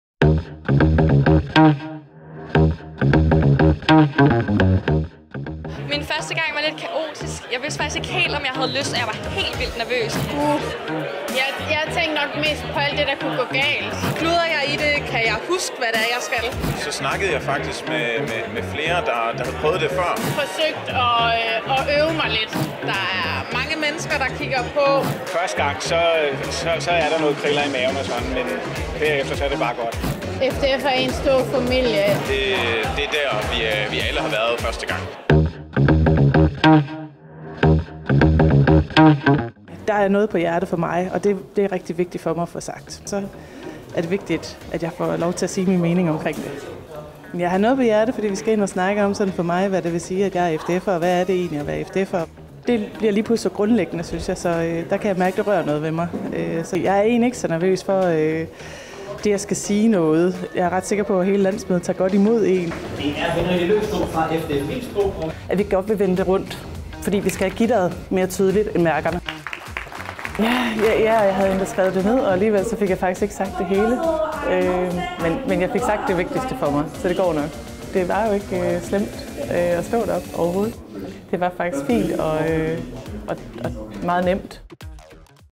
Den allerførste gang
Hør FDFere fortælle om, hvordan det er, den første gang.

Filmredaktion: Christian Damsgaard, Rikke Nord, Anders Ekström og Brian Karstensen